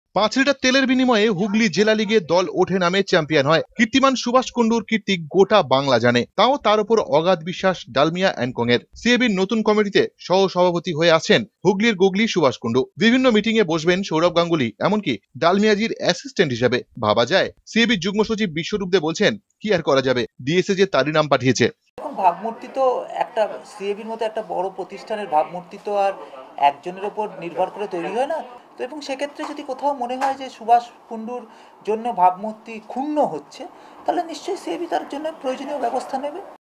CAB ELECTION: CORRUPTED SUBHASH KUNDU SET TO BECOME THE VICE-PRESIDENT OF CAB
CAB election got another controversial dimension as Subhash Kundu alias Mr.Corruption is set to become the Vice-President of CAB. It is said that, in Hooghly, football teams often get relegated and promoted in the district football through the transaction of oil. It is so unfortunate that CAB is all set to bring this ugly personality on the field of CAB administration. It is not that the ruling group did not have the better choices for that post as they have the likes of Biswarup Dey, who is a dead-honest person in all means. When asked, Biswarup said,"CAB is a huge organization and if we see that the image of CAB is getting mortificated for a single individual, we'll take necessary steps against him."